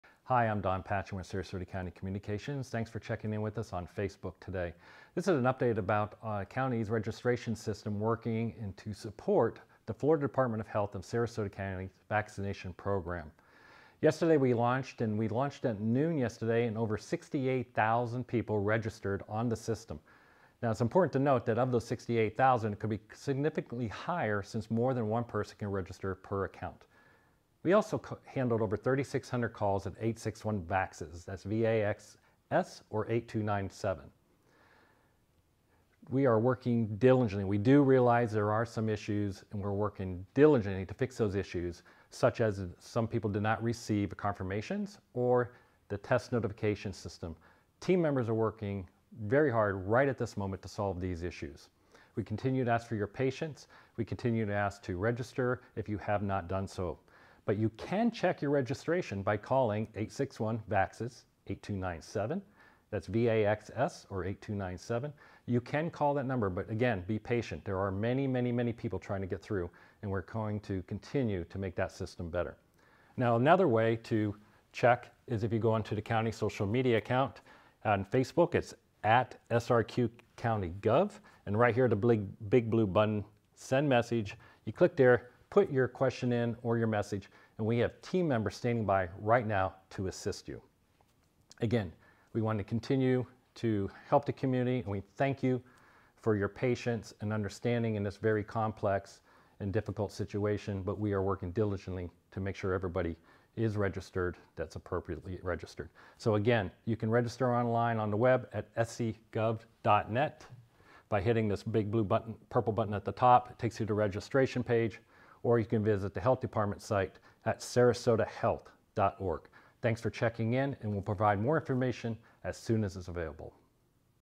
Vaccine Registration System Update: Jan. 21, 2021
✅ The new COVID-19 registration system launched yesterday at noon, and over 68,000 individuals have registered.

➡ The team is working diligently to correct issues like confirmations and the test system.

➡ Appointments for Friday's clinic have not been scheduled yet. We will share an update once we know the timeframe of those notifications later today.